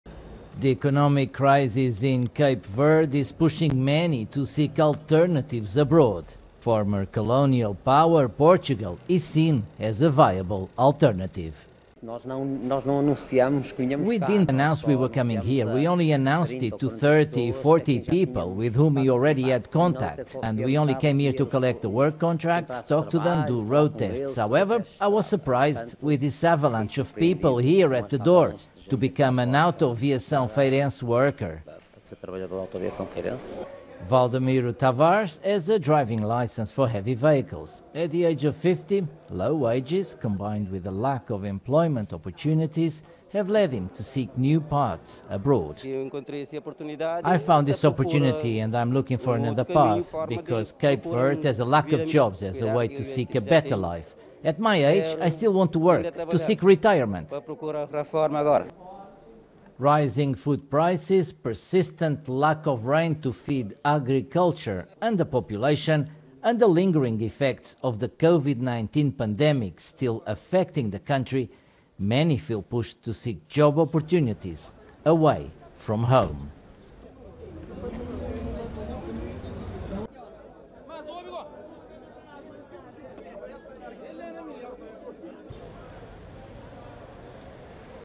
Cape Verdeans seek opportunities abroad
Faced with rising inflation at home many Cape Verdeans are seeking job opportunities abroad